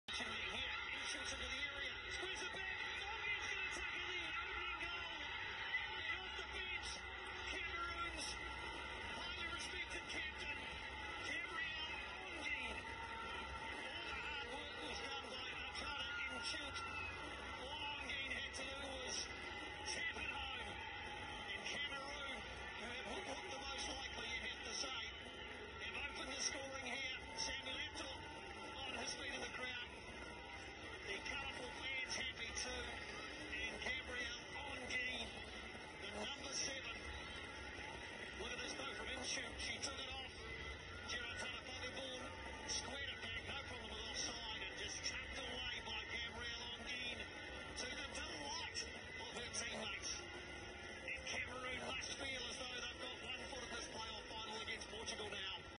BUT DE ABOUDI ONGUENE-CAMEROON vs THAÏLANDE_BARRAGES COUPE DU MONDE
EN NOUVELLE ZELANDE CE JOUR LE CAMEROUN S'IMPOSE FACE À LA THAÏLANDE GRACE À 2 BUTS DE ABOUCHOUCHOU ONGUENE ABOUDI